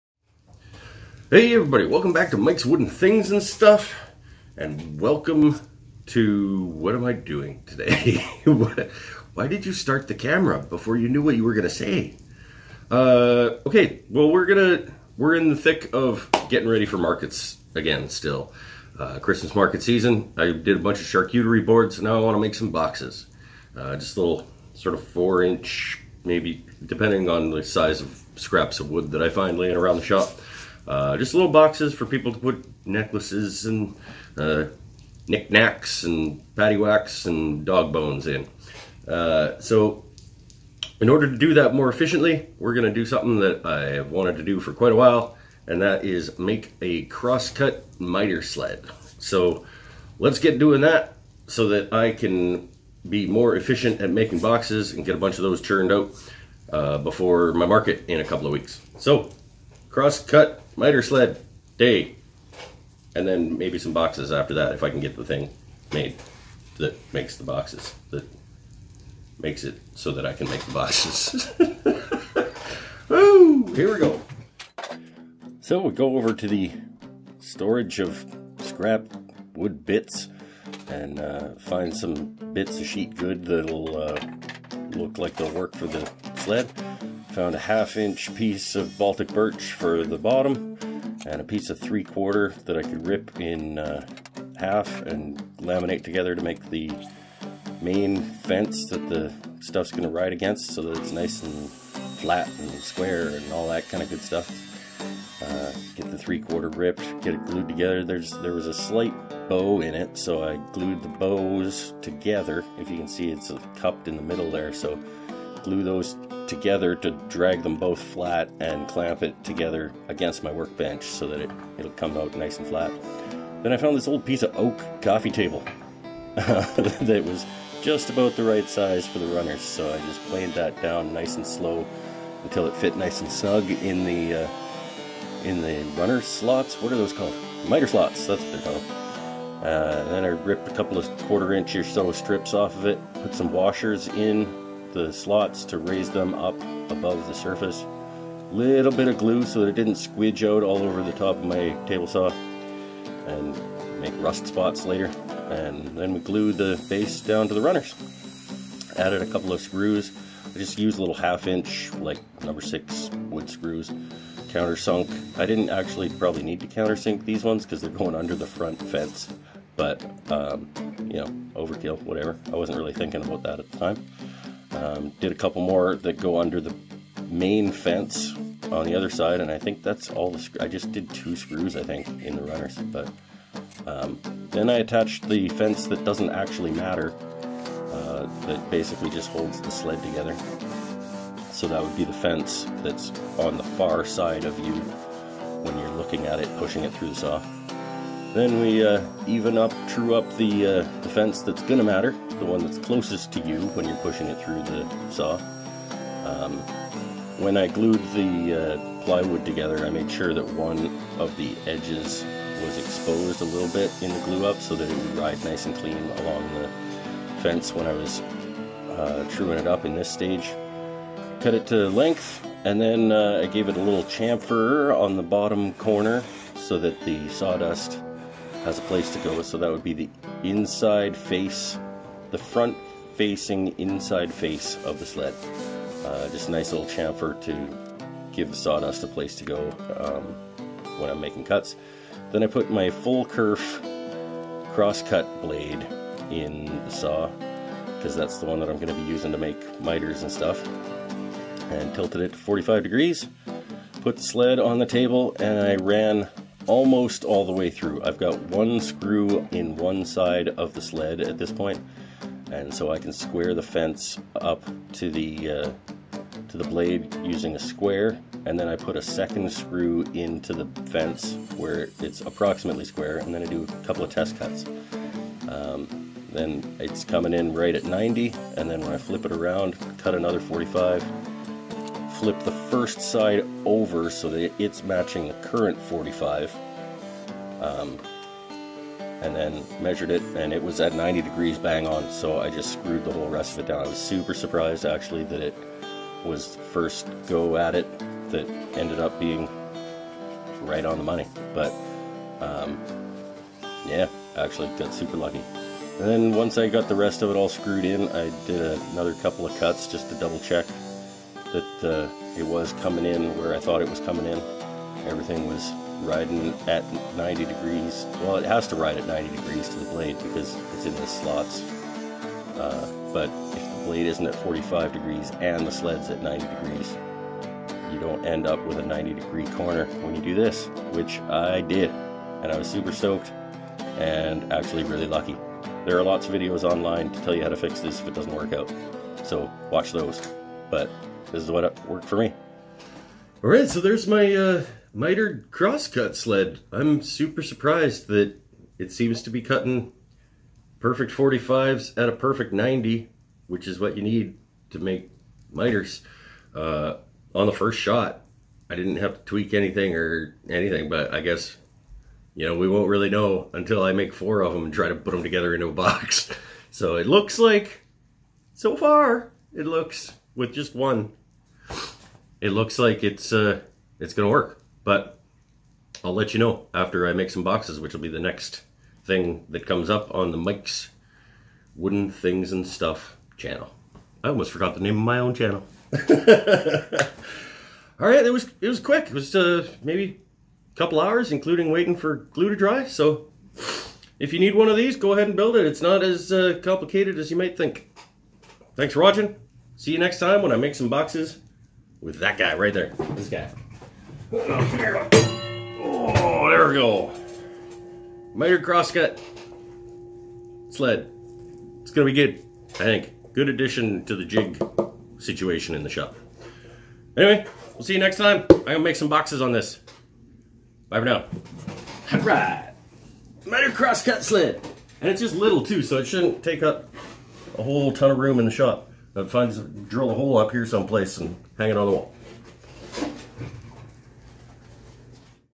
I Finally Made a Miter Crosscut Sled!
I needed to batch out a bunch of mitered boxes, so I decided to make something I've wanted for a long time: A miter crosscut sled.

Head over and “Like” my Facebook page!

Oh! And now I’m also on Instagram!